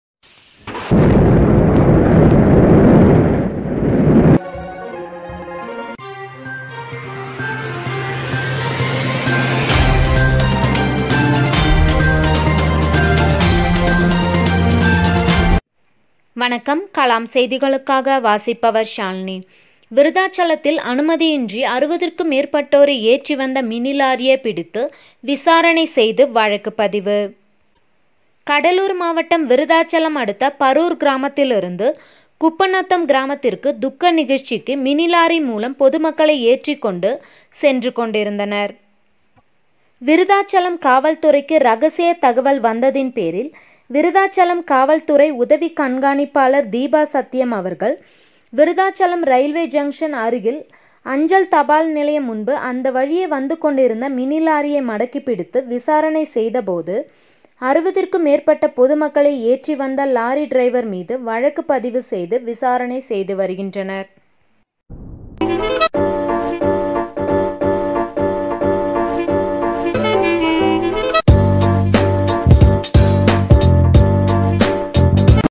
KALAM NEWS கடலூா் மாவட்டம் விருதாசலத்தில் 60 க்கும் மேற்பட்டோரை ஏற்றி வந்த மினி லாரியை பிட
KALAM NEWS
கடலூா் மாவட்டம்
விருதாசலத்தில்   60 க்கும் மேற்பட்டோரை ஏற்றி வந்த மினி லாரியை பிடித்து வழக்கு பதிவு